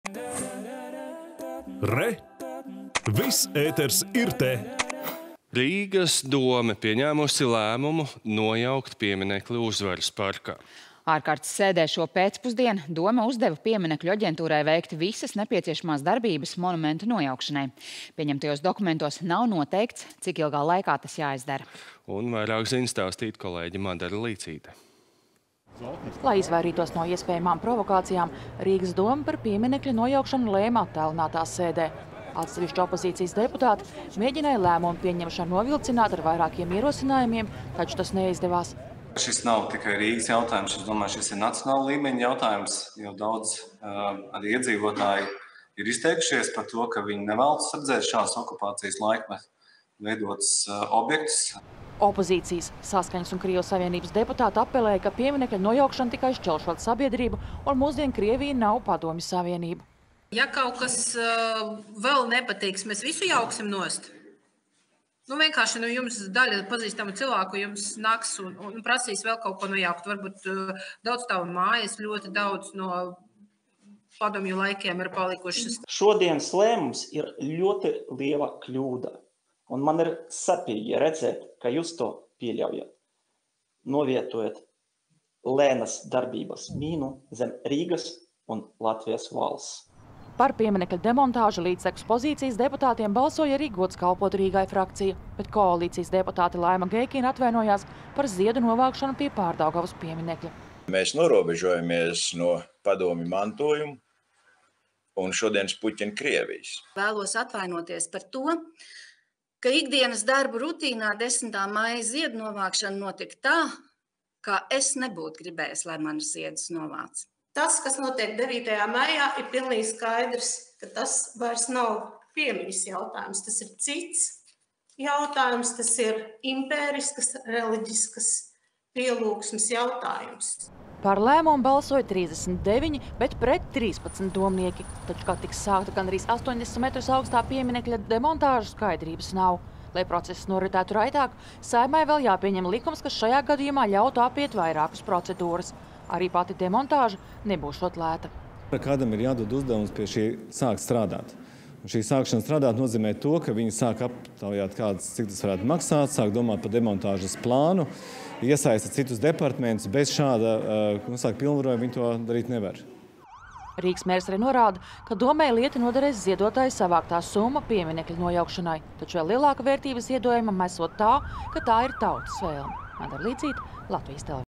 Rīgas dome uzdod nojaukt pieminekli Uzvaras parkā
Rīgas dome pieņēmusi lēmumu nojaukt pieminekli Uzvaras parkā. Ārkārtas sēdē šo pēcpusdien dome uzdeva Pieminekļu aģentūrai veikt visas nepieciešamās darbības monumenta nojaukšanai. Pieņemtajos dokumentos nav  noteikts, cik ilgā laikā tas ir jāizdara. Turpina Madara Līcīte.